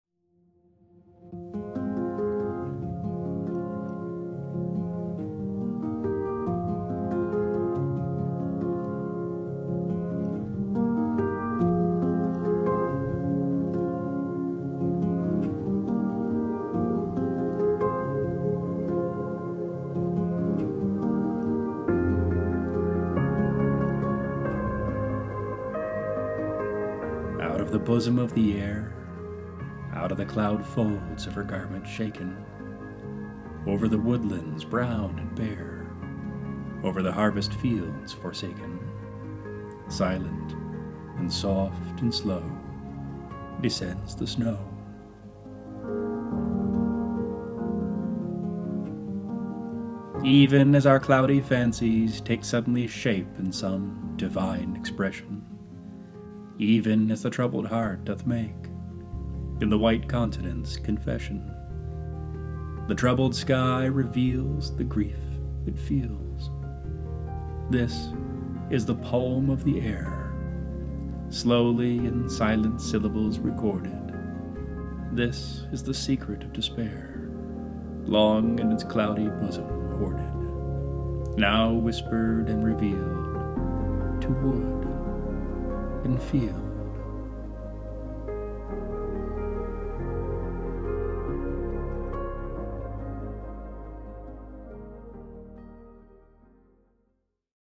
"Snow-flakes" by Henry Wadsworth Longfellow, read for Lit Out Loud by Benjamin Karl
I looked outside and said to myself "wow! I've got to capture this!" My wife agreed; in fact she insisted that I grab my cameras and head out into the yard before this beautiful miracle of nature passed us by.

Snow had recently fallen and was now slipping from the tree-tops, made heavy in the afternoon sun and dancing downward through its rays. The footage both inspired and accompanies this week's episode. Please enjoy Henry Wadsworth Longfellow's poem "Snow-flakes," both read and filmed by me, Benjamin Karl.

Lit Out Loud Podcast Season 01 Episode 04

"Snow-flakes" by Henry Wadsworth Longfellow is in the public domain in the United States and other countries. The associated video was filmed and edited by Benjamin Karl.